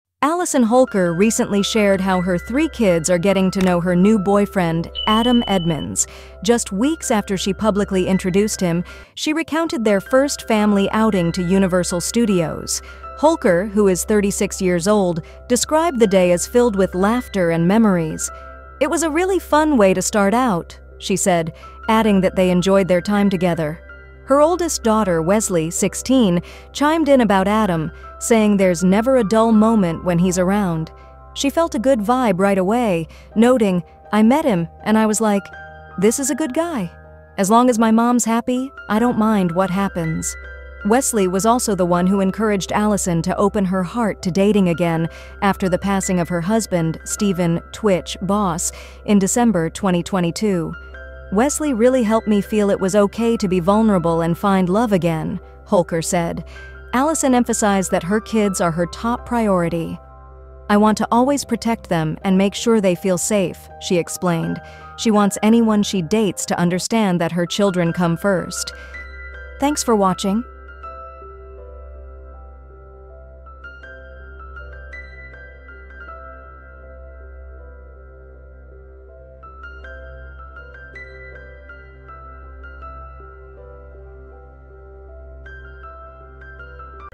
Allison Holker Shares How She Introduced Her New Boyfriend to Her Three Kids!
Allison Holker REVEALS How She Introduced New Boyfriend to Her Three Kids .Allison Holker opens up about her journey introducing her new boyfriend, Adam Edmunds, to her three kids. After a fun family outing at Universal Studios, her oldest daughter, Weslie, shared her positive impressions of Adam, emphasizing that he brings joy to their lives. As they navigate this new chapter, Holker prioritizes her children's feelings, ensuring they feel safe and loved. She reflects on the impact of her late husband, Stephen “tWitch” Boss, and how that love has shaped her readiness for this new relationship. Join us as we explore this heartfelt story of love, family, and moving forward.